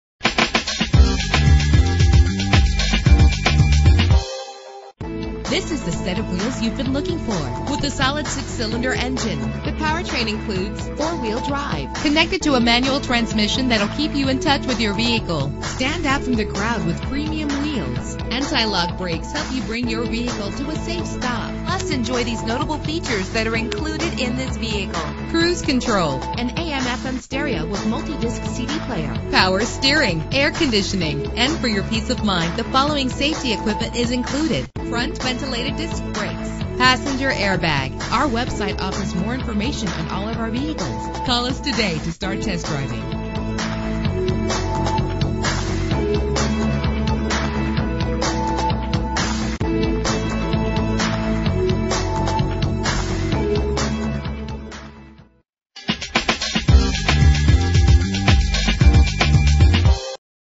2000 Jeep Wrangler - Whittier CA
Year: 2000
Make: Jeep
Model: Wrangler
Trim: Sport
Engine: 4.0 liter inline 6 cylinder 12 valve
Transmission: Manual
Color: Yellow
Mileage: 133697
Address:
11703 Carmenita Rd.